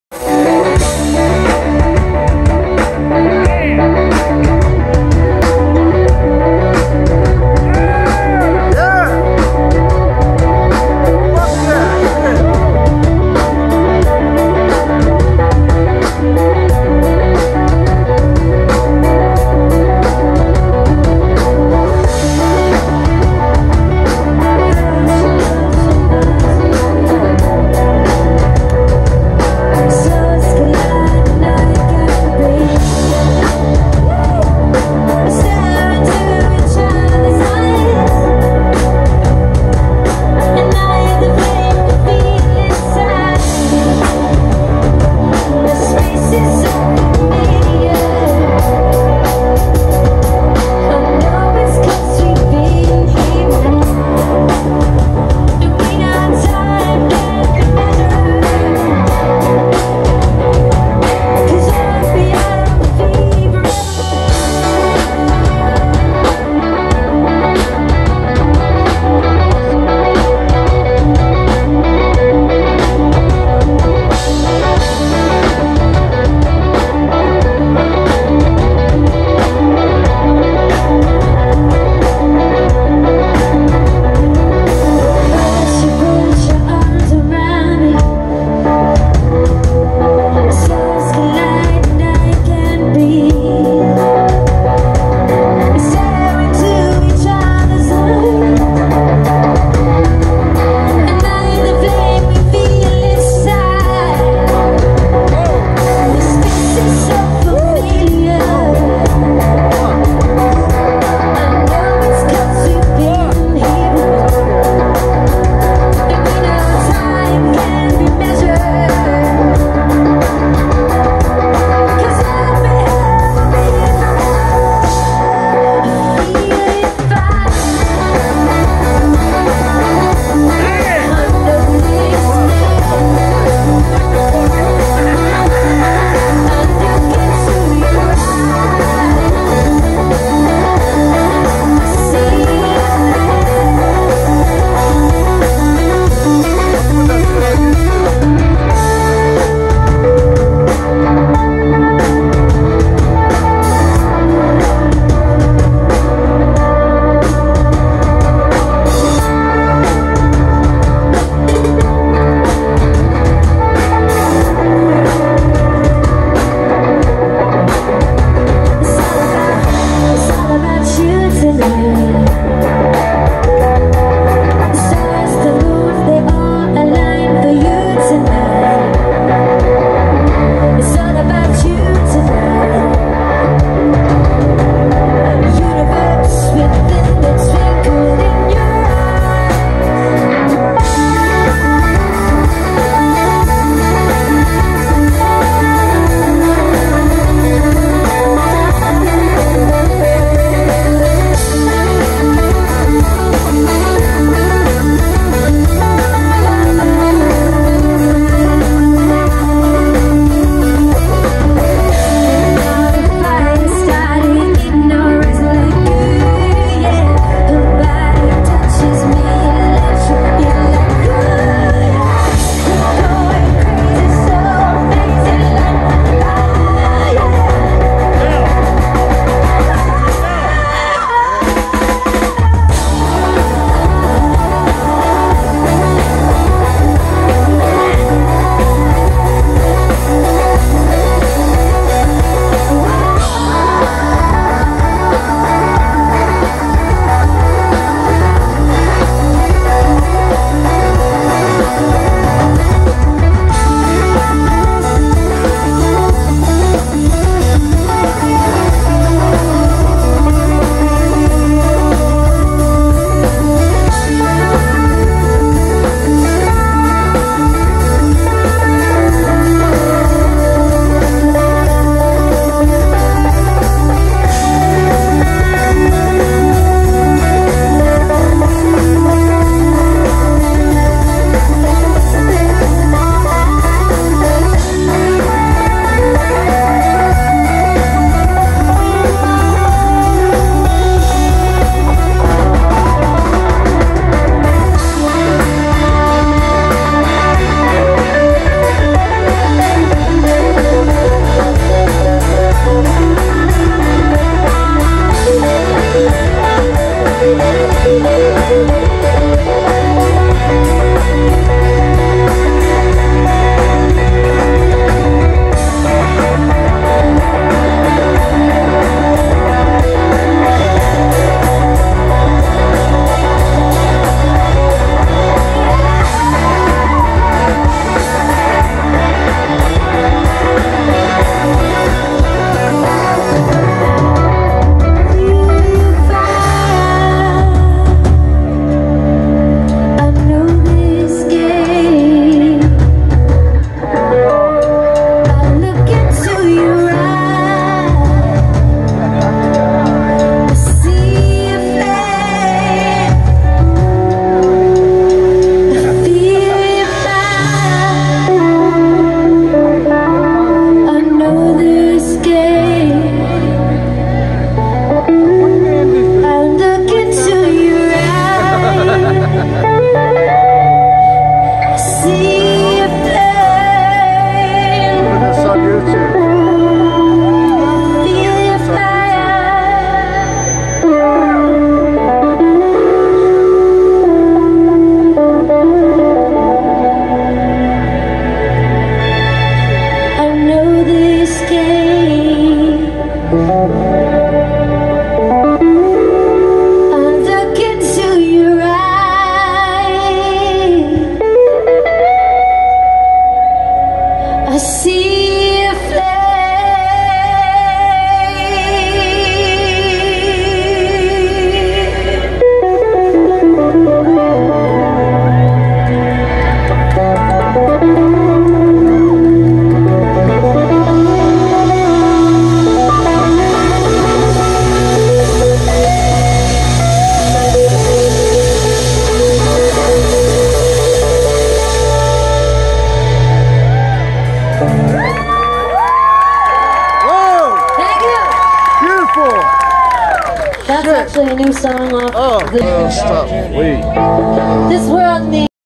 The Black Monkeys in Tarpon Springs Florida
Music at the hippie festival